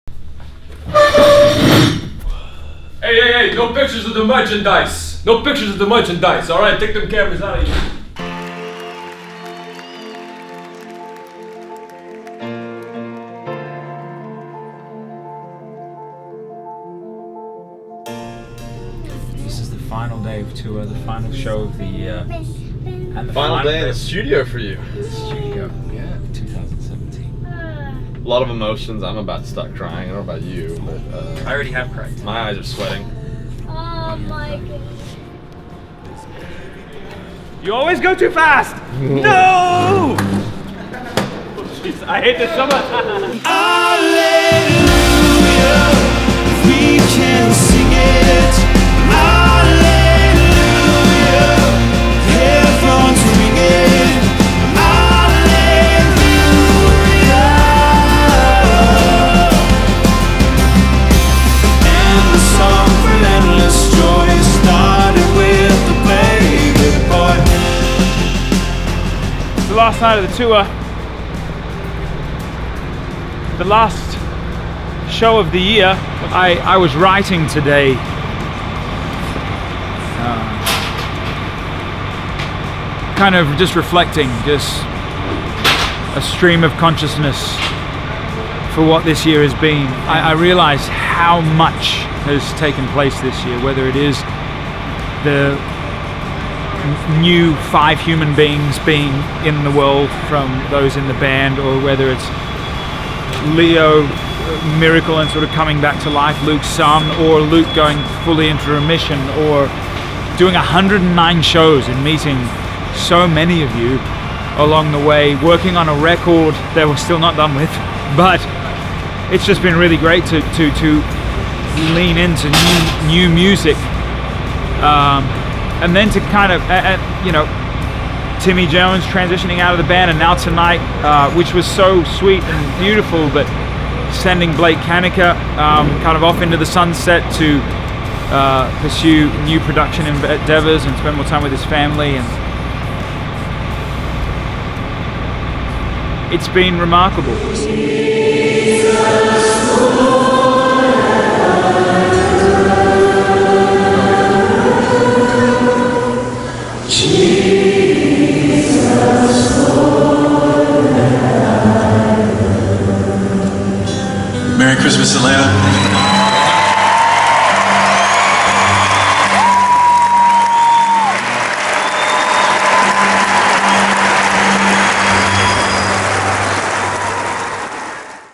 A Glorious Christmas 2017: Final Night of Tour [Behind The Scenes]
A year takes many twists and turns and for me, some of the twists were enjoyable but others, much more difficult.  I’m thankful for the memories of 2017, but I look forward to 2018 with great hope and anticipation of what it’ll be. There might be challenges, but 2017 taught me how to have joy in the midst of struggle so I’ll take that great joy with me everywhere I go in 2018. Happy New Year!